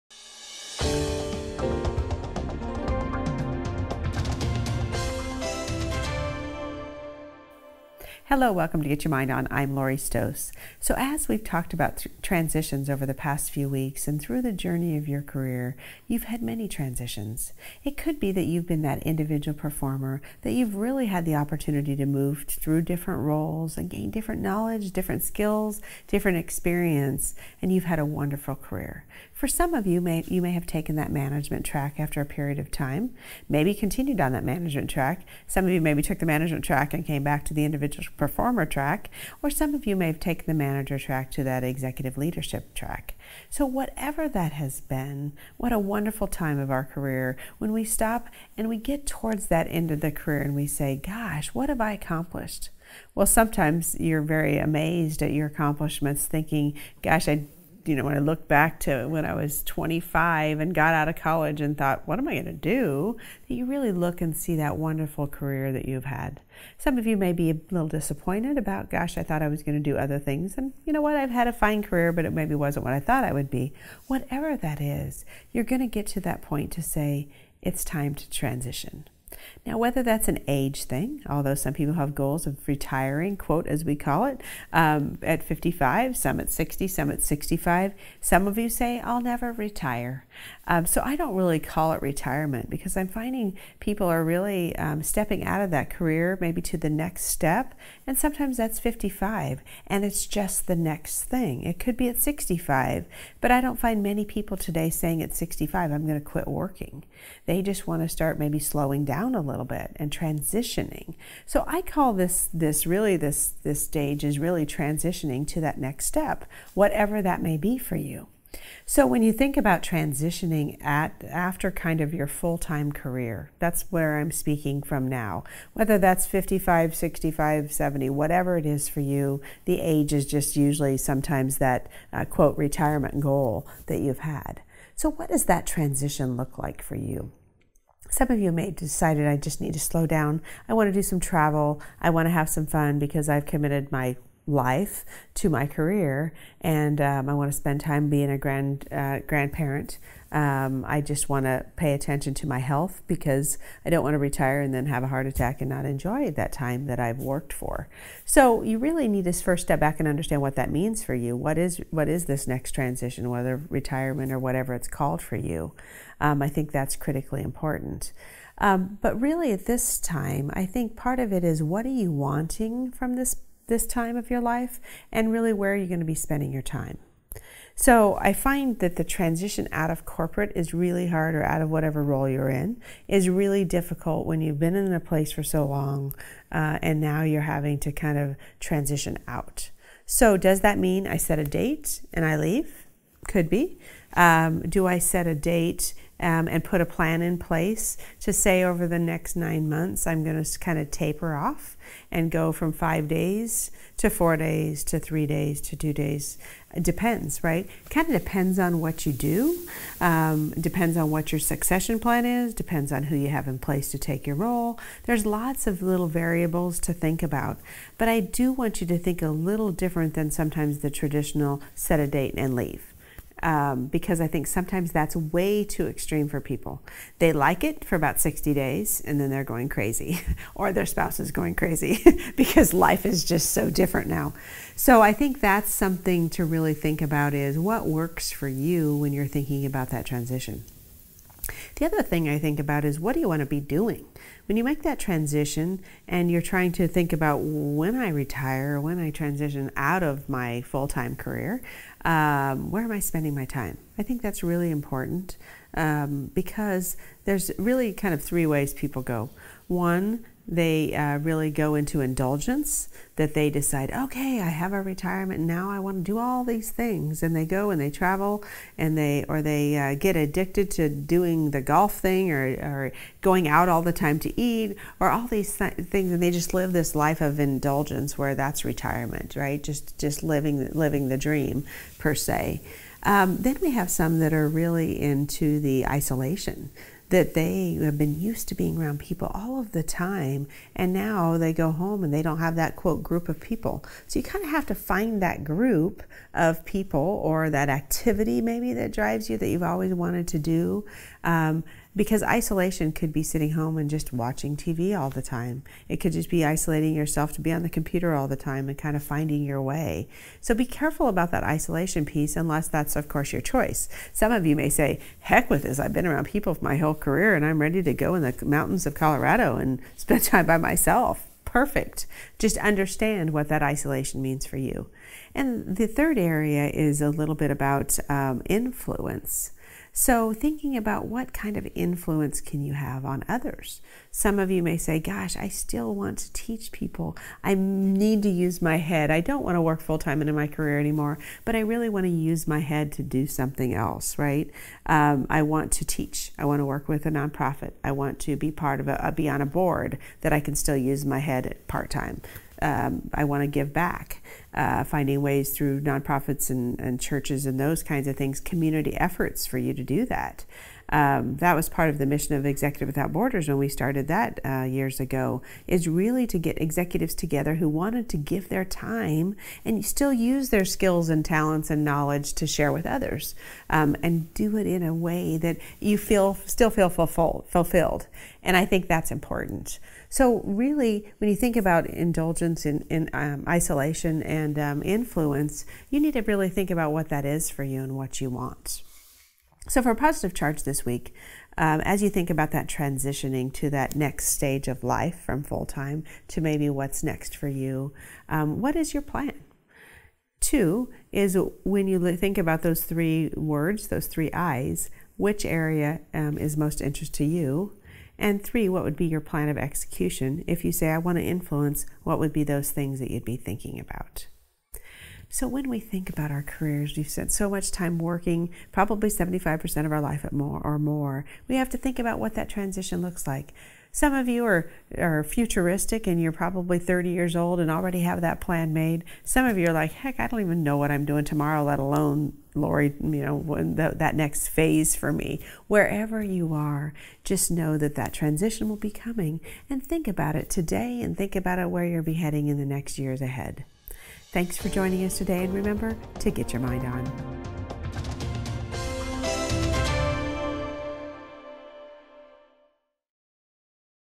Transitioning to the Next Stage
How have your transitions allowed you to grow?

Can you think of improvements that have occurred in your life through transitions?

What’s next for you?

When the time is right, you may be looking for other alternatives as your career is coming to a close. If you’ve decided that it’s time to make a change in your life, it’s important to analyze what that next stage is and how to successfully transition without overwhelming yourself. In this GET YOUR MIND ON, I help you to evaluate your next stage and how it can allow you to enjoy a healthy and happier life.